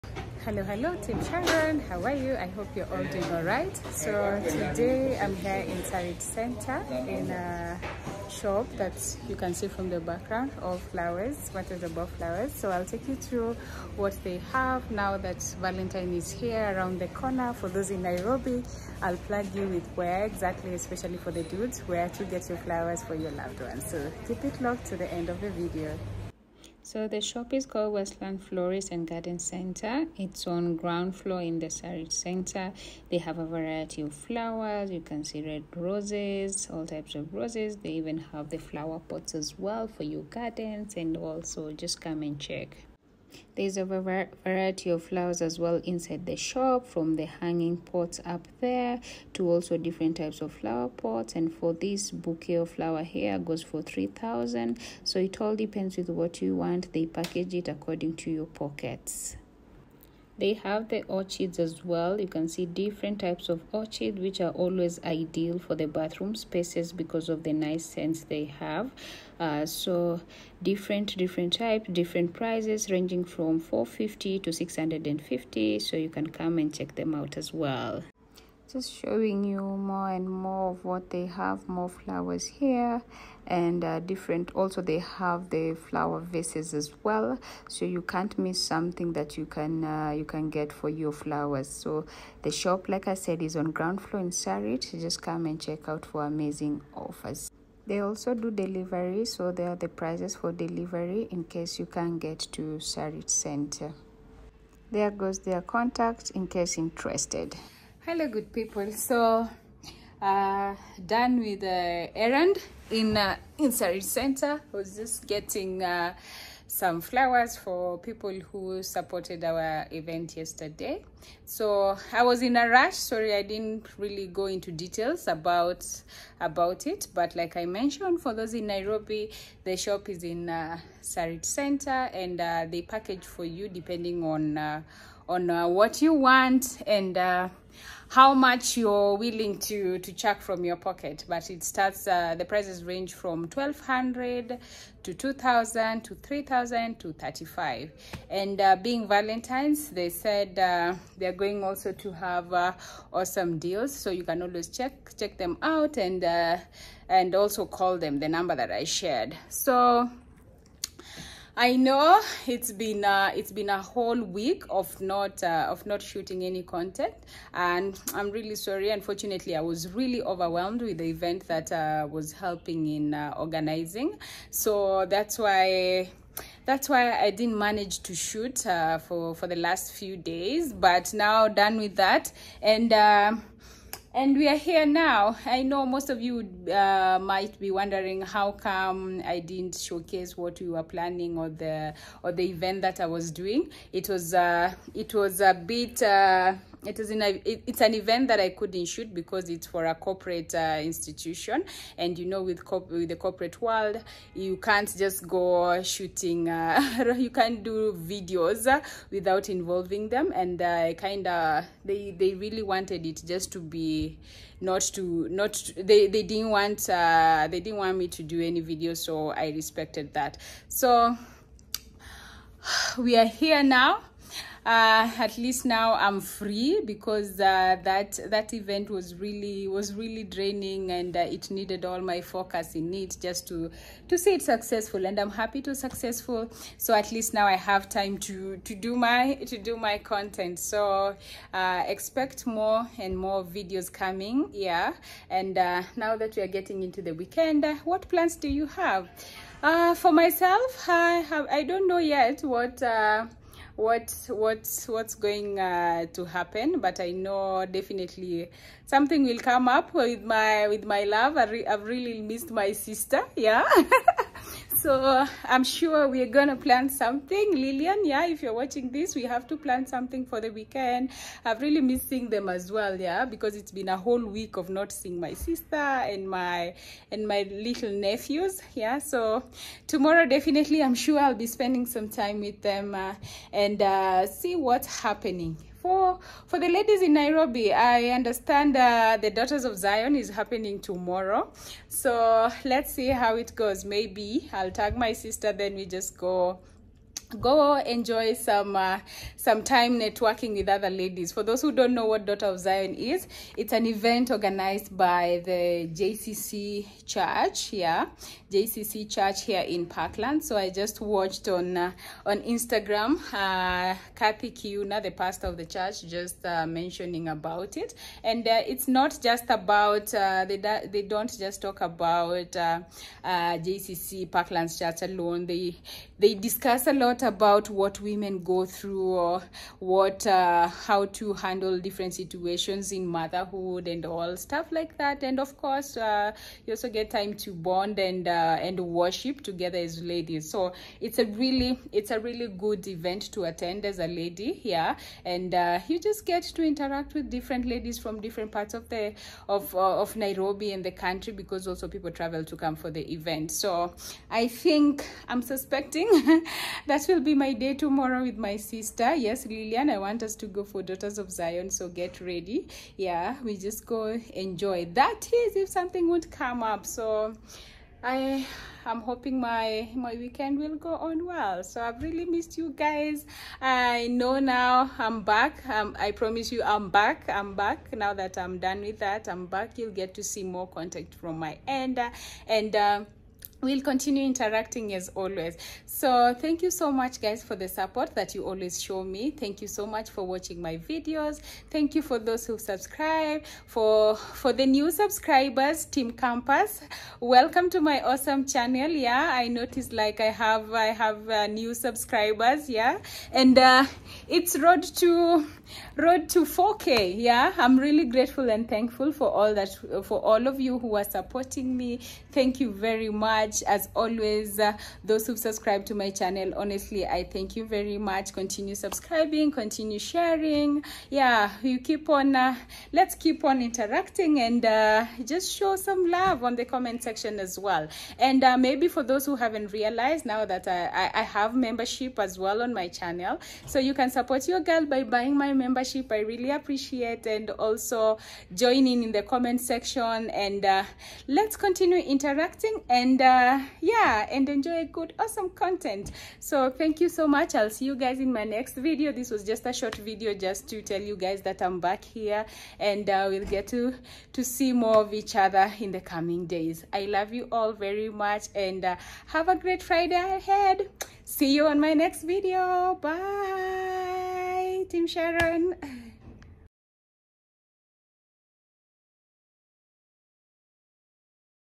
A DAY AS A FLORIST IN NAIROBI,KENYA
Visited the FLOWER shop in Westlands Nairobi and got to find out what they have.